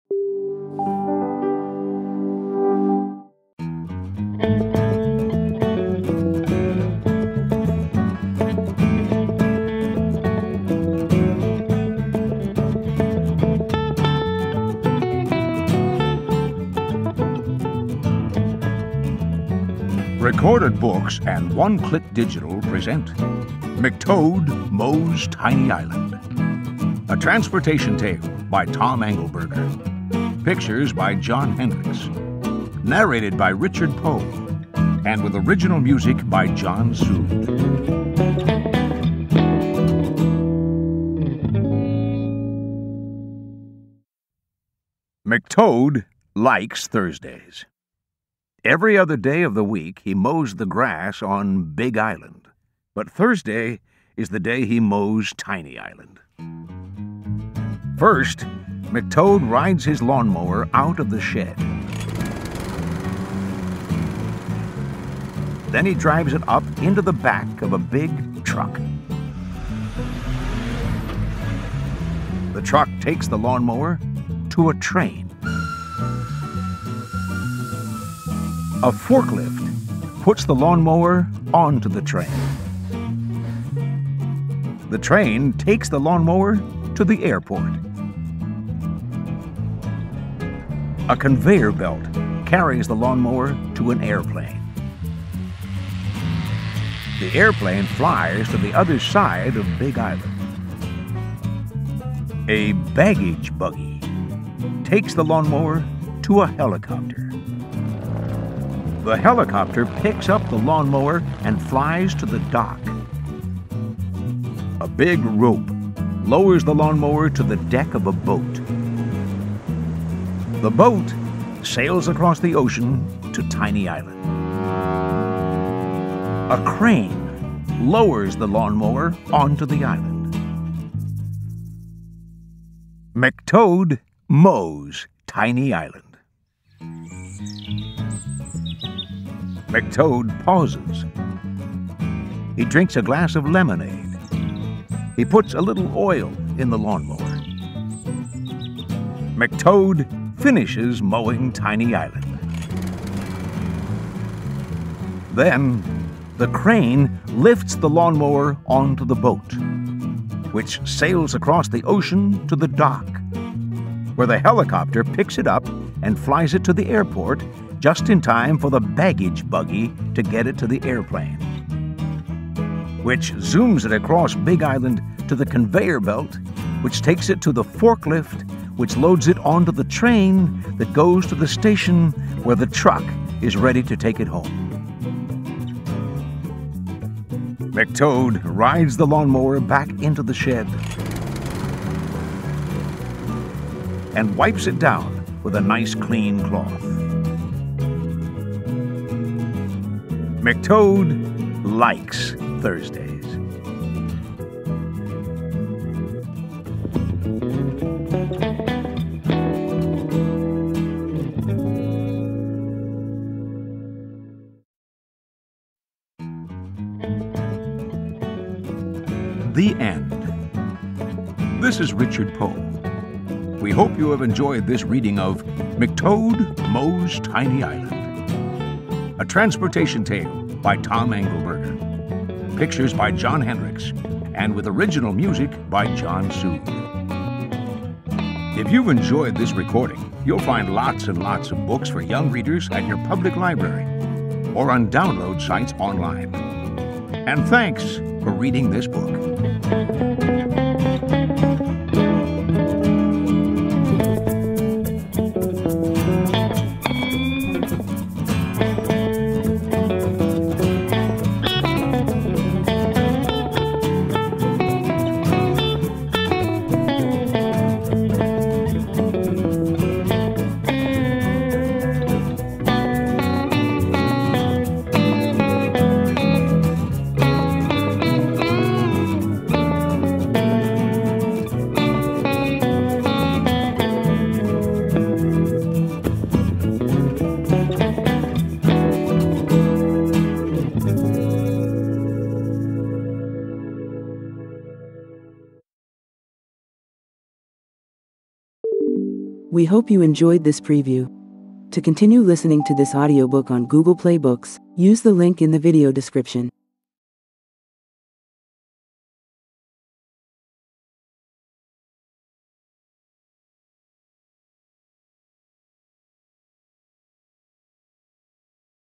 McToad Mows Tiny Island by Tom Angleberger · Audiobook preview
McToad Mows Tiny Island
Authored by Tom Angleberger
Narrated by Richard Poe

0:00 Intro
0:03
4:58 Outro

BOOK DESCRIPTION

McToad likes Thursdays. Why? Because on every other day of the week, McToad mows Big Island, but on Thursdays, McToad mows Tiny Island. To do so, he puts his mower on the back of a truck, which drives to a train, which goes to a helicopter, which flies to a boat, which uses a crane to put the lawn mower onto Tiny Island. There McToad mows and drinks some lemonade, and before you know it, it's time to turn around and go back home. But first, the mower has to get lifted by a crane, to get put back on a boat, which is lifted by a helicopter, and . . . well . . . you get the idea.

Tom Angleberger was a journalist before becoming an author-illustrator. His works include the Origami Yoda series, Fake Mustache, and Horton Halfpott or, the Fiendish Mystery of Smugwick Manor; or, the Loosening of M'Lady Luggertuck's Corset. In 2014 his title's Princess Labelmaker to the Rescue: An Origami Yoda Book and Emperor Pickletime Rides the Bus made The New York Times Best Seller List.

AUDIOBOOK DETAILS

Language: English
Publisher: Recorded Books
Published on: September 1, 2015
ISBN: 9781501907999
Duration: 0 hr, 5 min
Genres: Juvenile Fiction / Animals / Frogs & Toads, Juvenile Fiction / Humorous Stories, Juvenile Fiction / Transportation / General